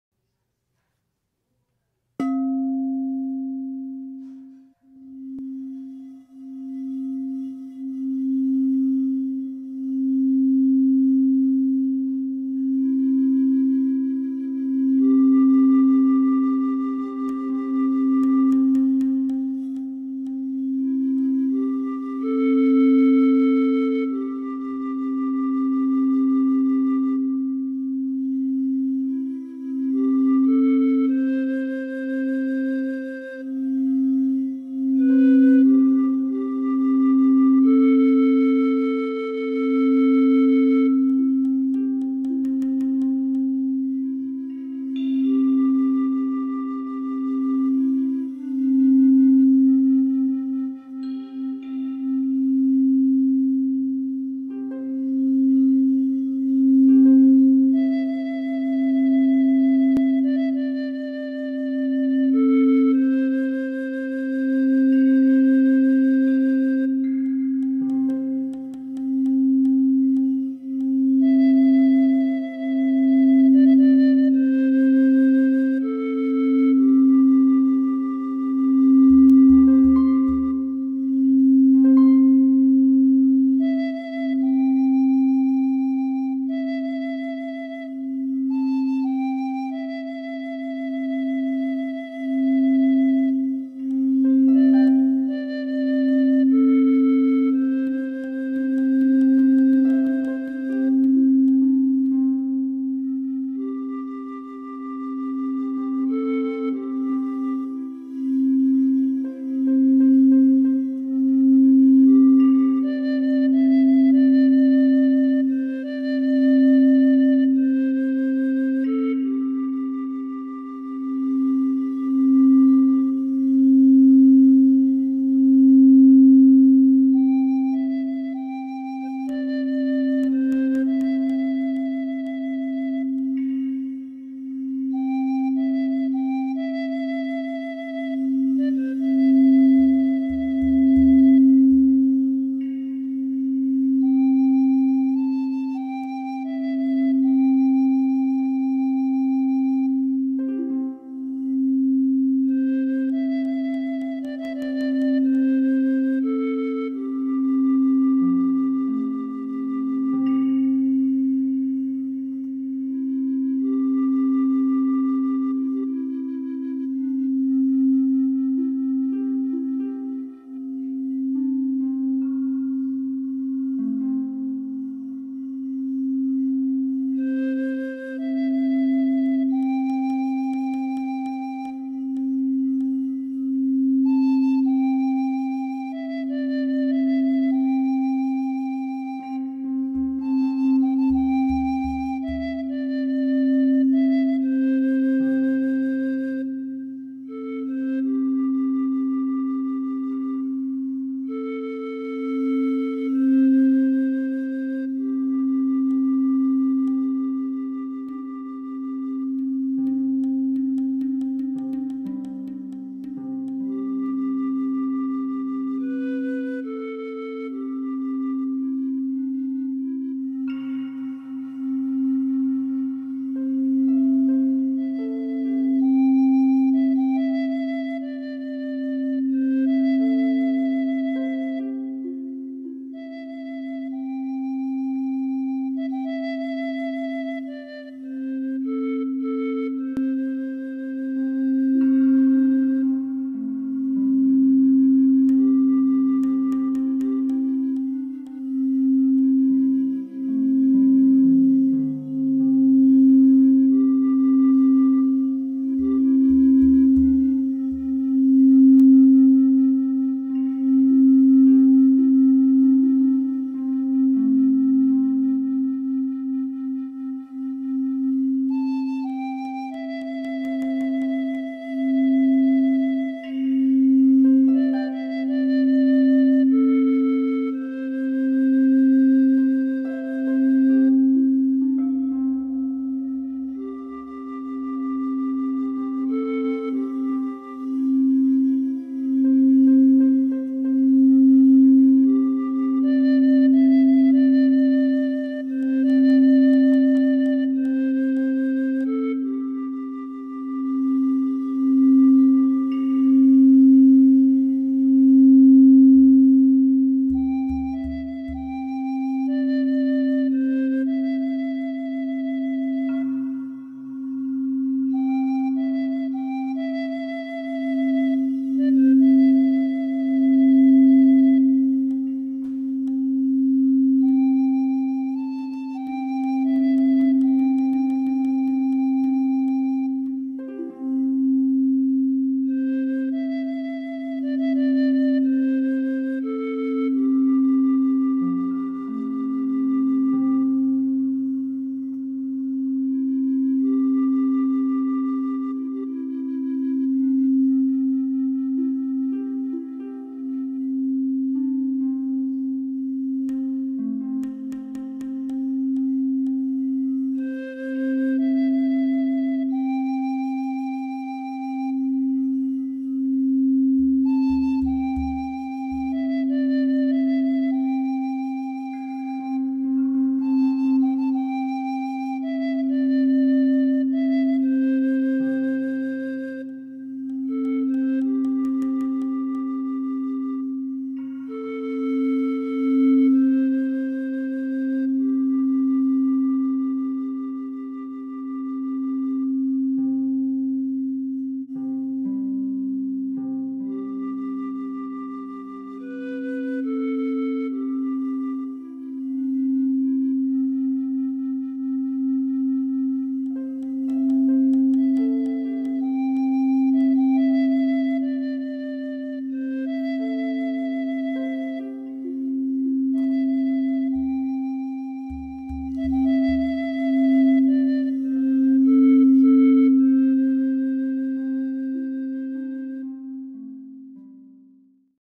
Crystal singing bowl with flute and harp
Video performed by Claudio Senna Venzke (photos and crystal singing bowl) and Alyson Wendhausen (musics). Use to meditate when you need to connect to your basis (Basic Chakra). Photos taken in Boston area (USA) in Fall, 2013.